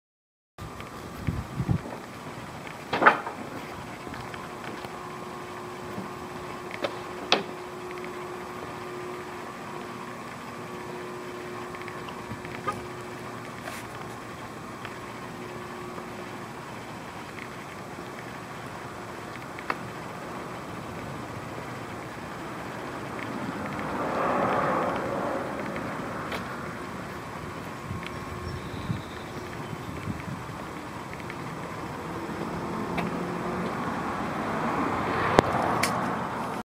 Achat | Vente Caravane de voyage | Bullet 230 BHS l 2014, stock KB-035, vue de l'extérieur
Vidéo vue de l'extérieur d'une Caravane de voyage | Bullet 230 BHS l 2014, stock KB-035, disponible chez Roulottes Lupien